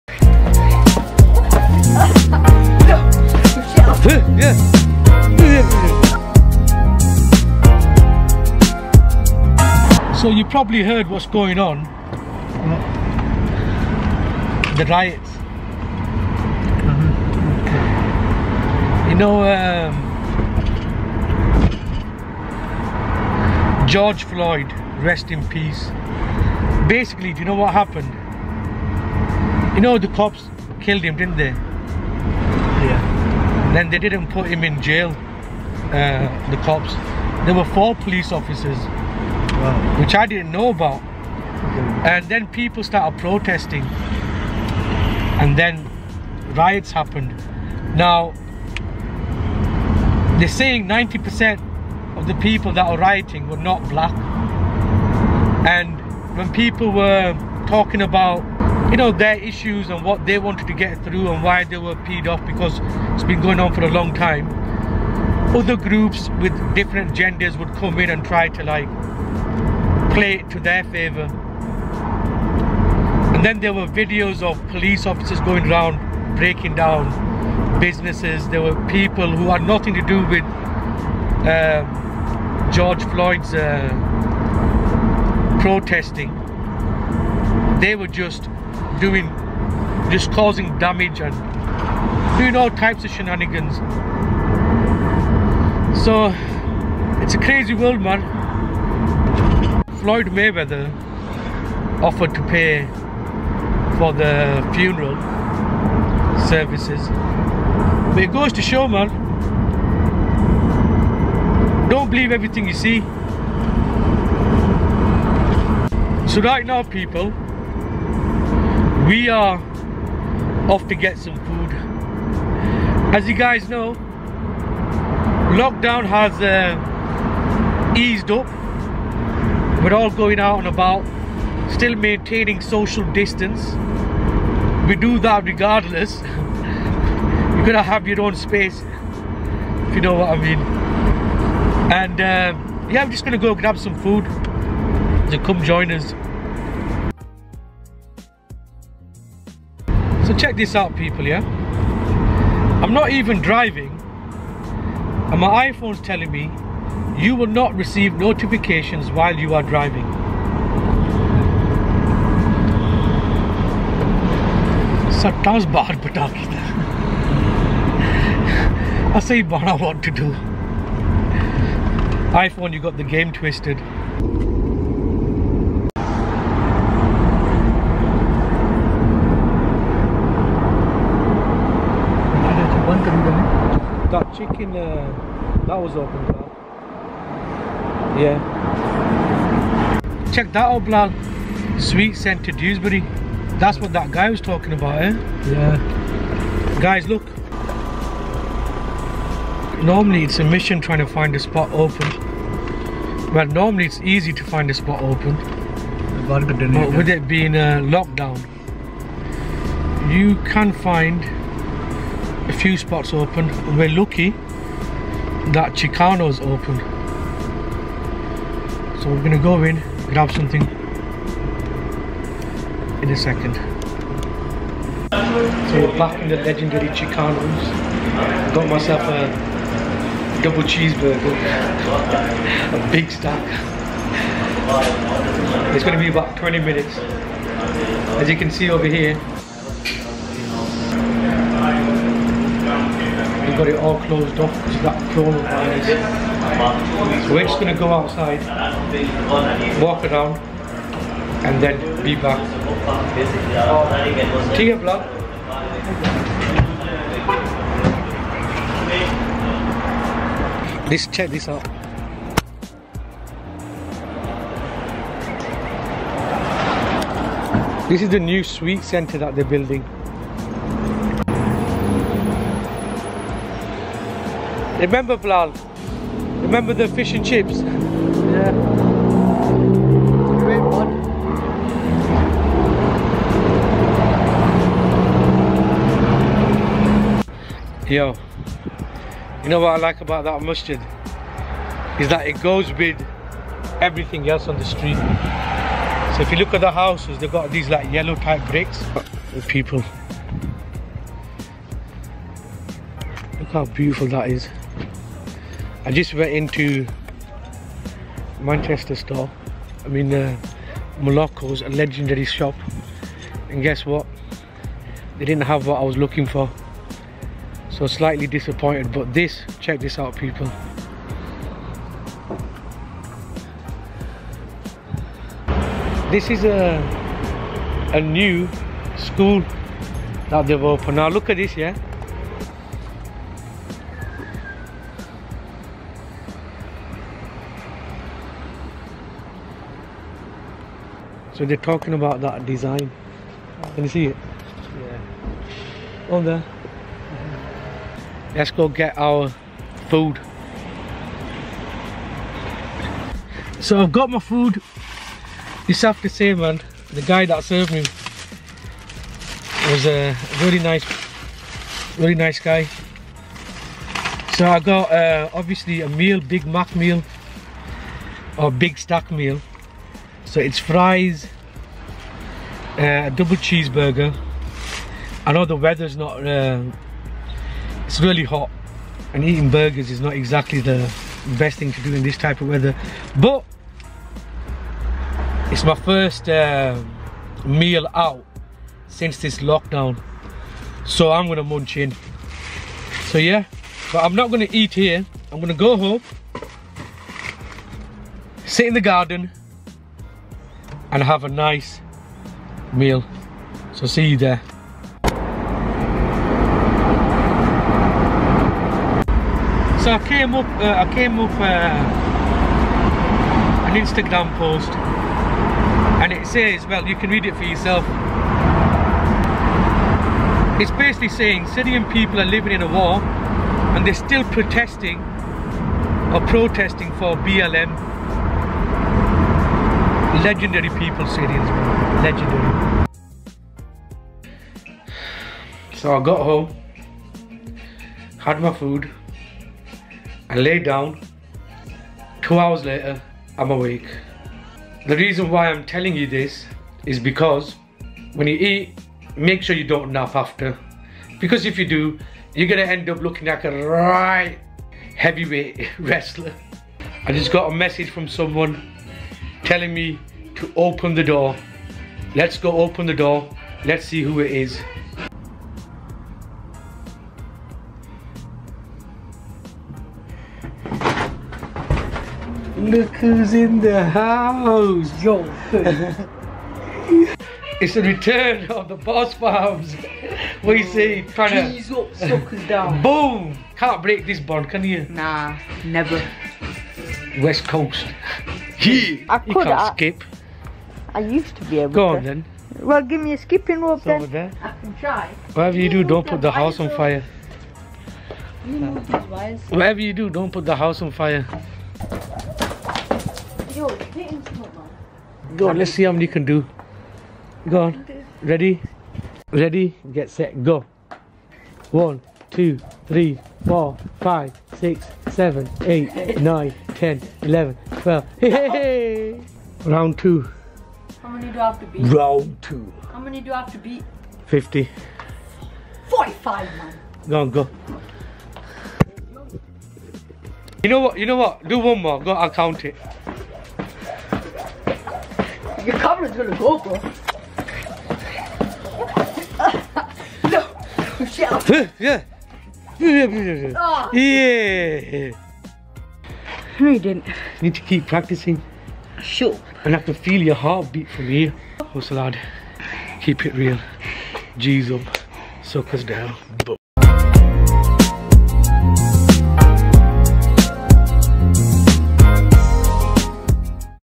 BEAUTIFUL DEWSBURY MOROCCAN INSPIRED MASJID ZAKARIA (LARGEST GEOMETRIC ART IN WHOLE OF EUROPE)
Hey yo boss famz

paid a visit to dewsbury and saw this beautiful building that i a masjid inspired by moroccan art really was a sight to see, loved it.

Plus grabbed some food and had to talk about what is happening in the world